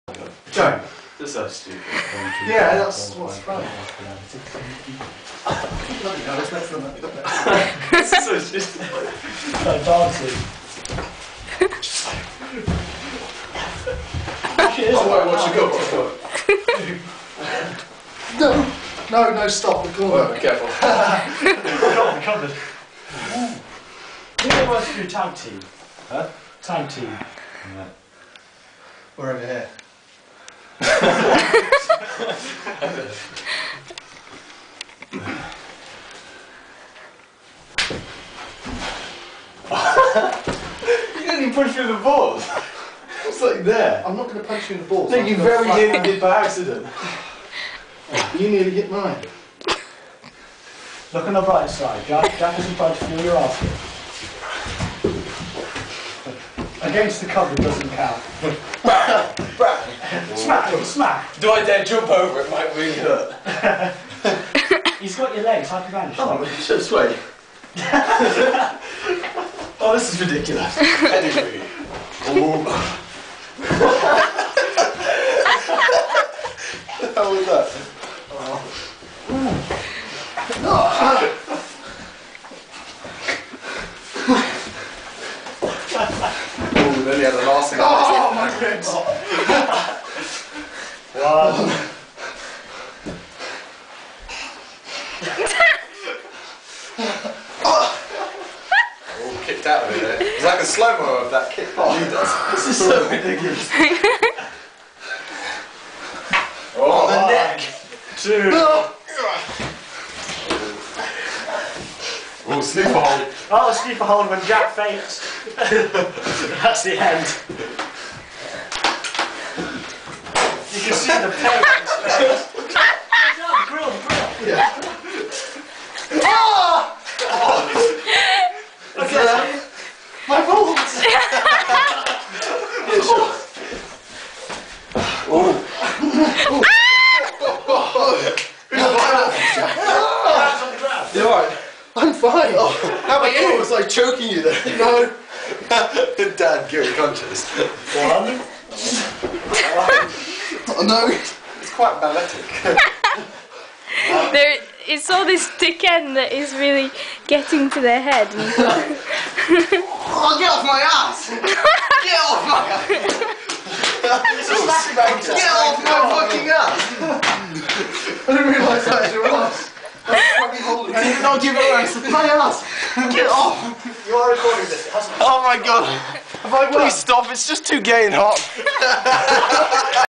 Flat Wrestling at Uni
just a bit of friendly play wrestling! lol, with a comic phrase at the end